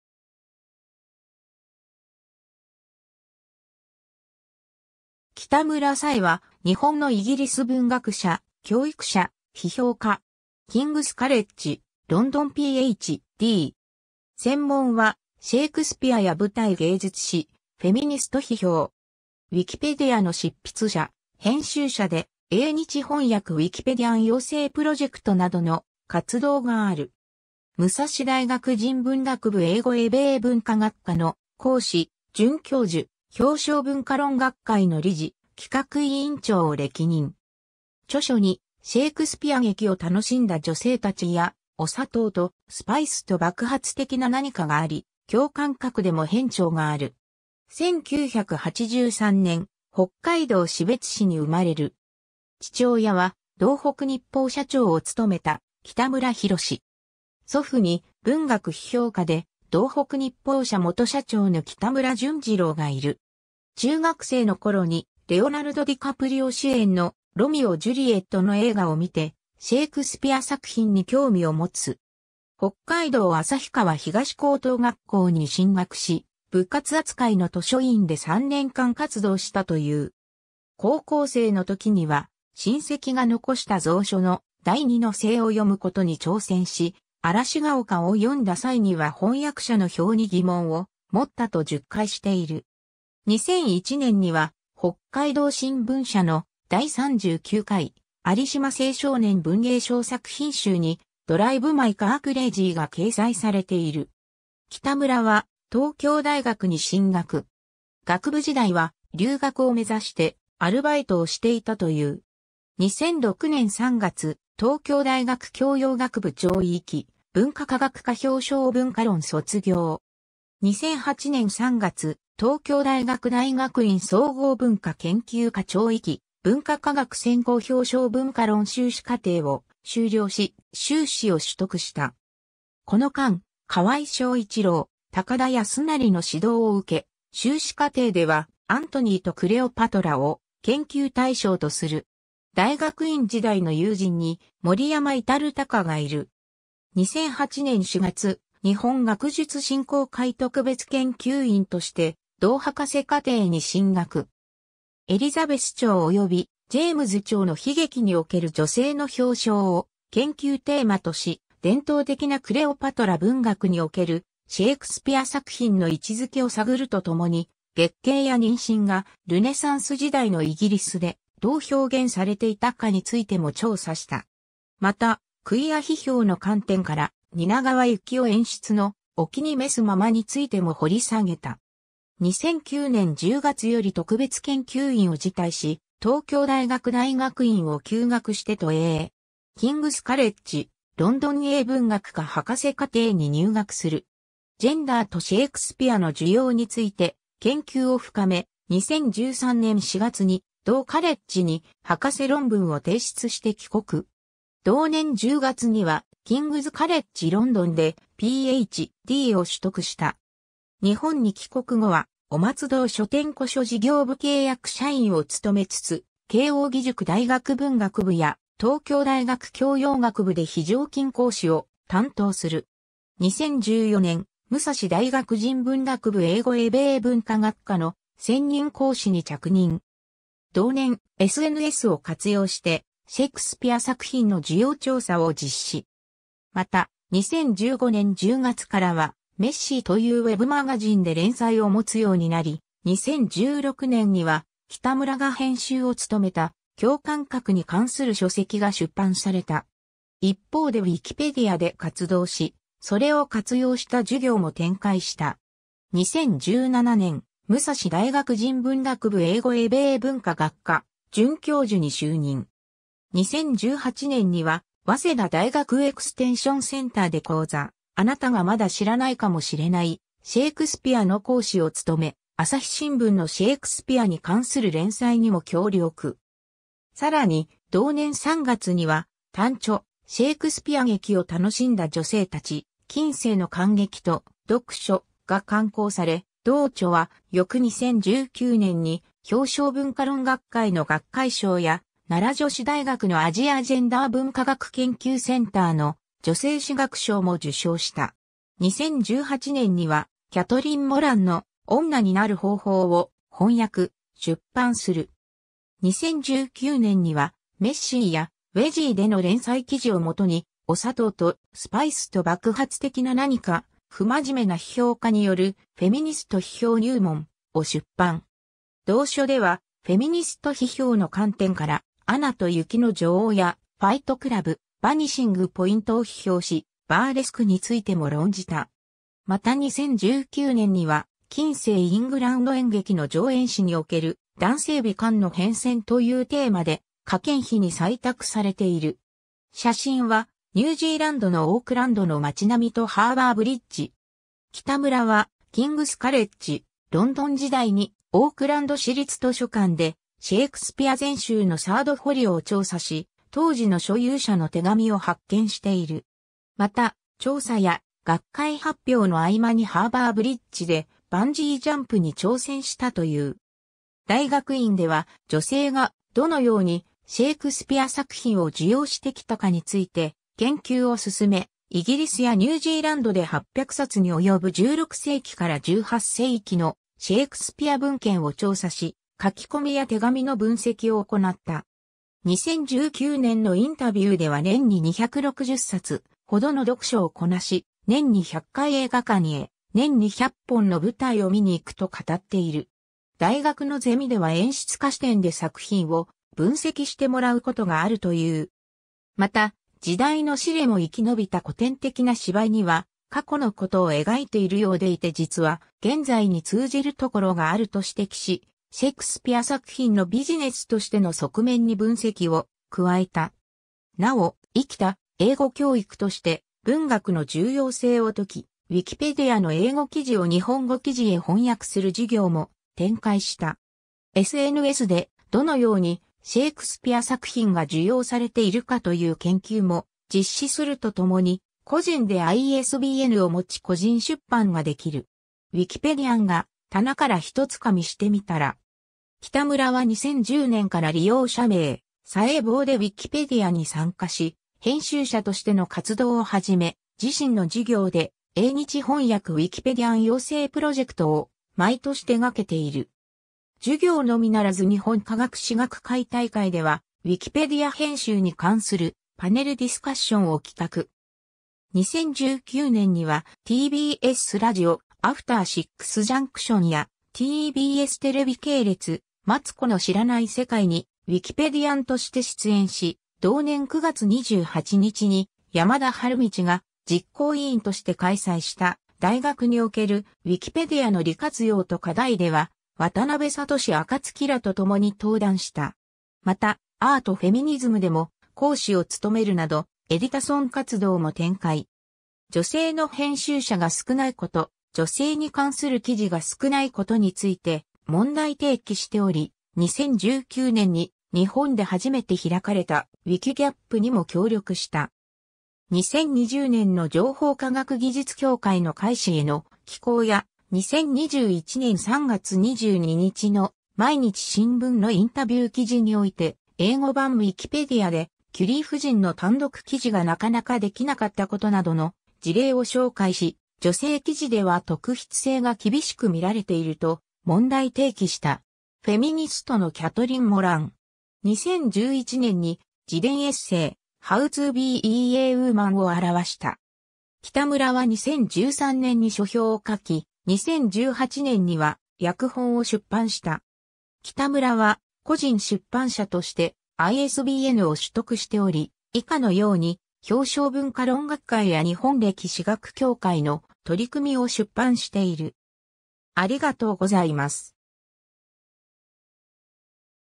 北村紗衣
修士（学術）取得者
学士（教養）取得者
北村 紗衣（きたむら さえ、1983年〈昭和58年〉4月12日 - ）は、日本のイギリス文学者、教育者、批評家。
キングス・カレッジ・ロンドンPh.D。
専門はシェイクスピアや舞台芸術史、フェミニスト批評。
ウィキペディアの執筆者・編集者（ウィキペディアン）で、「英日翻訳ウィキペディアン養成プロジェクト」などの活動がある。
武蔵大学人文学部英語英米文化学科の講師、准教授、表象文化論学会の理事、企画委員長を歴任。
著書に『シェイクスピア劇を楽しんだ女性たち』や『お砂糖とスパイスと爆発的な何か』があり、共感覚でも編著がある。
 1983年、北海道士別市に生まれる。
父親は道北日報社長を務めた北村浩史。
祖父に文学批評家で道北日報社元社長の北村順次郎がいる。
中学生の頃にレオナルド・ディカプリオ主演の『ロミオ+ジュリエット』の映画を見て、シェイクスピア作品に興味を持つ。
 北海道旭川東高等学校に進学し、部活扱いの図書委員で3年間活動したという。
高校生の時には親戚が残した蔵書の『第二の性』を読むことに挑戦し、『嵐が丘』を読んだ際には翻訳者の評に疑問を持ったと述懐している。
2001年には、北海道新聞社の第39回有島青少年文芸賞作品集に「Drive my car crazy」が掲載されている。
 北村は東京大学に進学。
学部時代は留学を目指してアルバイトをしていたという。
2006年3月、東京大学教養学部超域文化科学科表象文化論卒業。
2008年3月、東京大学大学院総合文化研究科超域文化科学専攻表象文化論修士課程を修了し、修士（学術）を取得した。
この間、河合祥一郎、高田康成の指導を受け、修士課程では『アントニーとクレオパトラ』を研究対象とする。
大学院時代の友人に森山至貴がいる。
 2008年4月、日本学術振興会特別研究員（DC1）として同博士課程に進学。
「エリザベス朝及びジェームズ朝の悲劇における女性の表象」を研究テーマとし、伝統的なクレオパトラ文学におけるシェイクスピア作品の位置付けを探るとともに、月経や妊娠がルネサンス時代のイギリスでどう表現されていたかについても調査した。
また、クィア批評の観点から、蜷川幸雄演出の『お気に召すまま』についても掘り下げた。
 2009年10月より特別研究員を辞退し、東京大学大学院を休学して渡英。
キングス・カレッジ・ロンドン英文学科博士課程に入学する（東京大学大学院は2012年9月に退学）。
ジェンダーとシェイクスピアの受容について研究を深め、2013年4月に同カレッジに博士論文を提出して帰国。
同年10月にはキングズ・カレッジ・ロンドンでPh.D（博士号）を取得した。
日本に帰国後は雄松堂書店古書事業部契約社員を務めつつ、慶應義塾大学文学部や東京大学教養学部で非常勤講師を担当する。
 2014年、武蔵大学人文学部英語英米文化学科の専任講師に着任。
同年、SNSを活用してシェイクスピア作品の受容調査を実施。
また、2015年10月からは『messy』というWebマガジンで連載を持つようになり（後に掲載メディアは『wezzy』に移行）、2016年には北村が編集を務めた共感覚に関する書籍が出版された。
一方でウィキペディアで活動し、それを活用した授業も展開した（節「ウィキペディアでの活動」も参照）。
 2017年、武蔵大学人文学部英語英米文化学科准教授に就任。
2018年には早稲田大学エクステンションセンターで講座「あなたがまだ知らないかもしれないシェイクスピア」の講師を務め、朝日新聞のシェイクスピアに関する連載にも協力。
さらに同年3月には、単著『シェイクスピア劇を楽しんだ女性たち ―近世の観劇と読書―』が刊行され、同著は翌2019年に表象文化論学会の学会賞や、奈良女子大学のアジア・ジェンダー文化学研究センターの「女性史学賞」も受賞した。
 2018年にはキャトリン・モランの『女になる方法』を翻訳・出版する。
2019年には『messy』や『wezzy』での連載記事をもとに、『お砂糖とスパイスと爆発的な何か ― 不真面目な批評家によるフェミニスト批評入門 ―』を出版。
同書ではフェミニスト批評の観点から『アナと雪の女王』や『ファイト・クラブ』、『バニシング・ポイント』を批評し、バーレスクについても論じた。